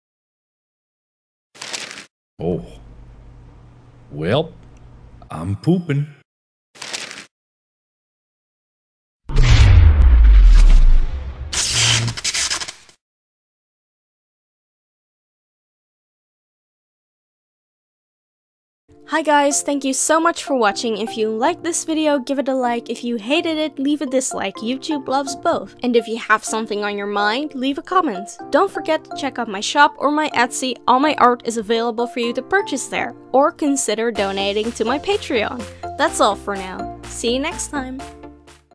.:Markiplier's... poopin':. MARKIPLIER ANIMATED | Pan-tastique
Just released: The teaser trailer for my upcoming animated pilot! Watch it today!

This is gonna get demonetized. But 30-second videos don't get ads anyway so EYYY

I don't know why I constantly come up with the weirdest kinds of animations when watching Markiplier. It's a curse :""D

I'm a freelance artist and animator using YouTube as a means to build my portfolio!